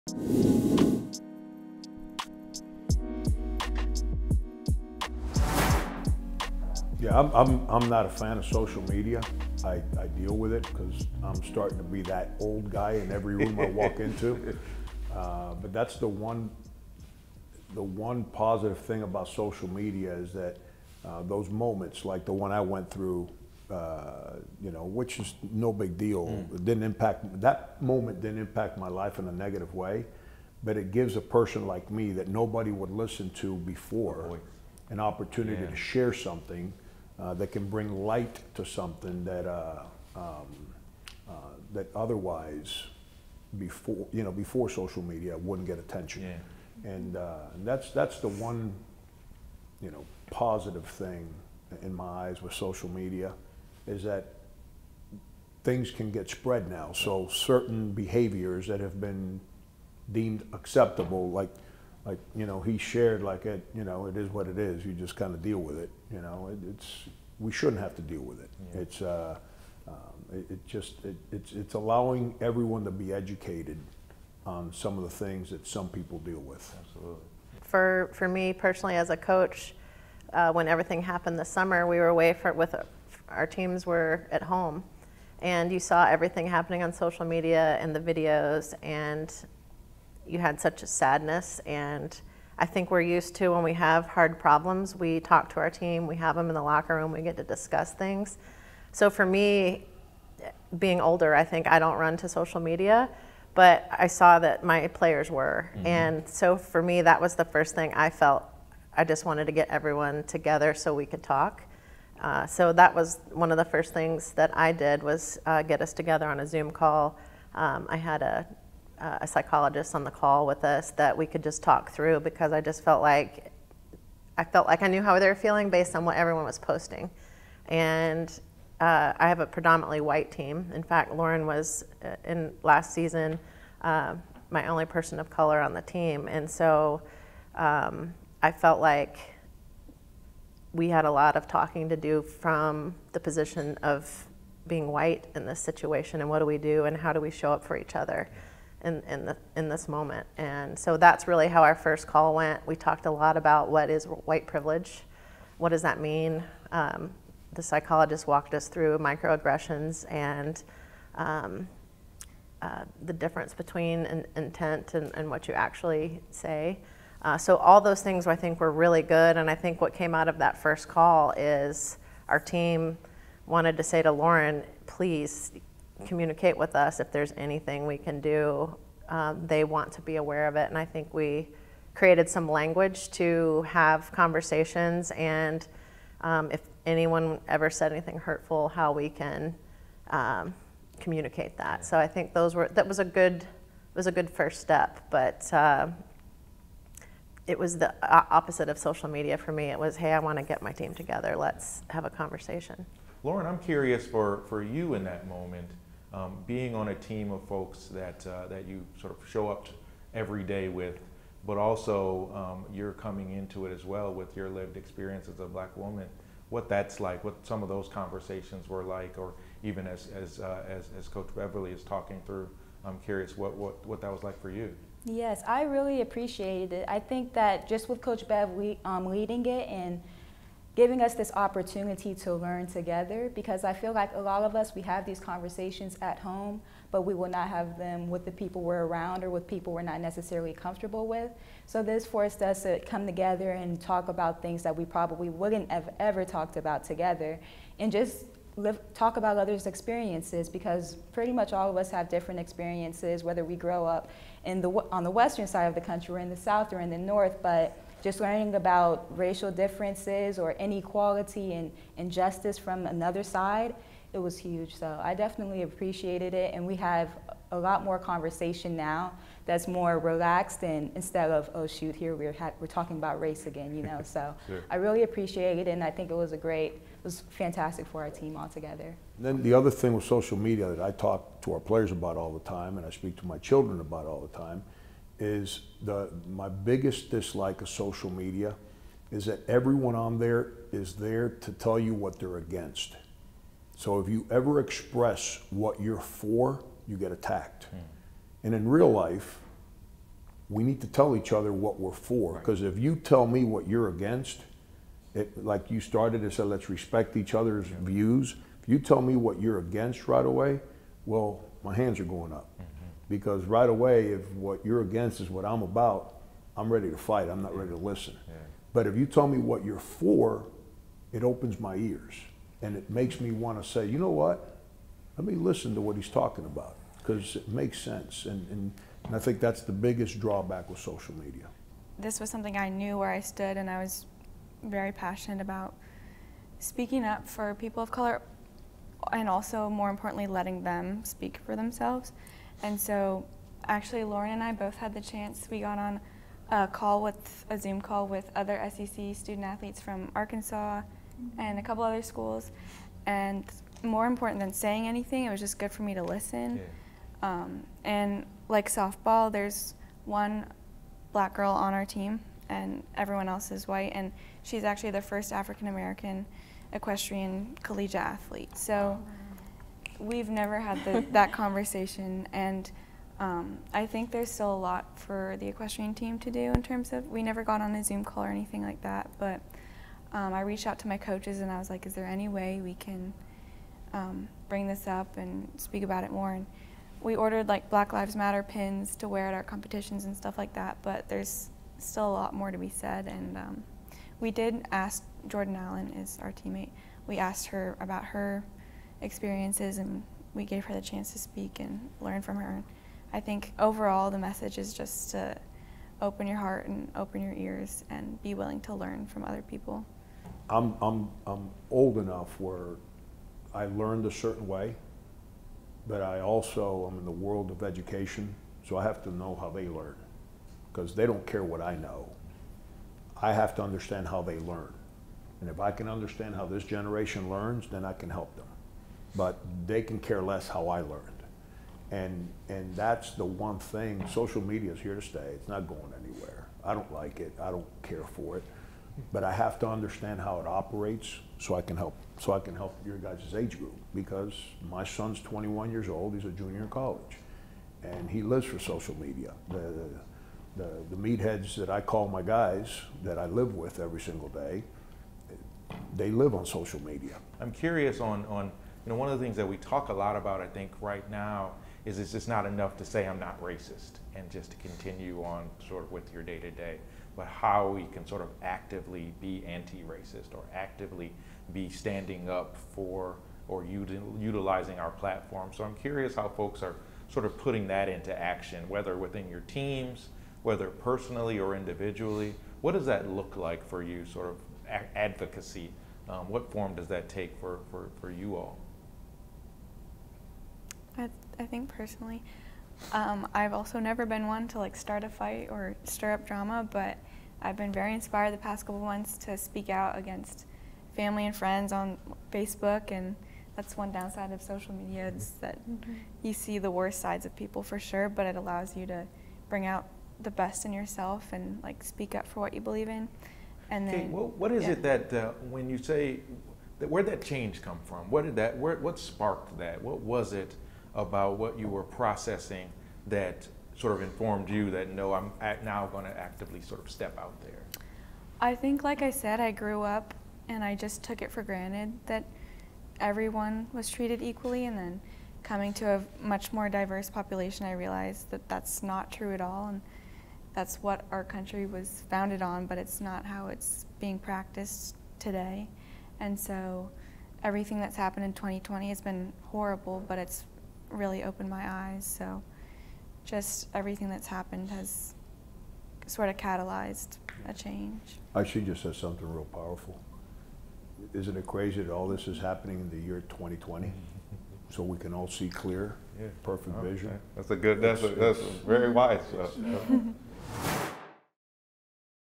Diversity and Inclusion Roundtable, I've Got Your Back
University of South Carolina coaches and student-athletes came together with UofSC Vice President for Diversity, Equity and Inclusion Julian Williams to talk about racial and social injustice in America.